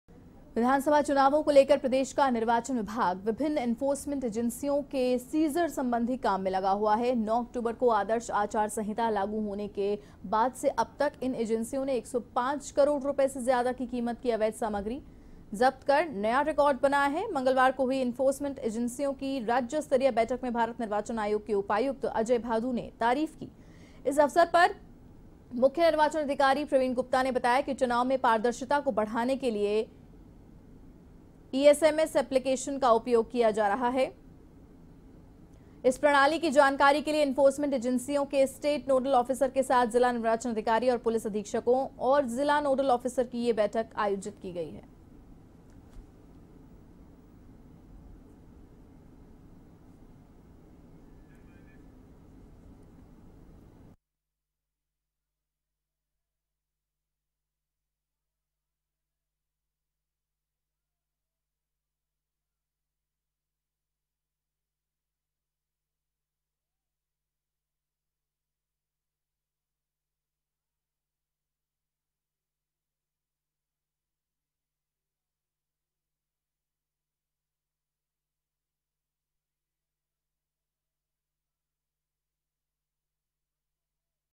आचार सहिंता लागू होने के बाद 105 करोड़ की अवैध सामग्री जब्त, निर्वाचन अधिकारी ने दी जानकारी
आचार सहिंता लागू होने के बाद 105 करोड़ की अवैध सामग्री जब्त, निर्वाचन अधिकारी प्रवीण गुप्ता ने दी जानकारी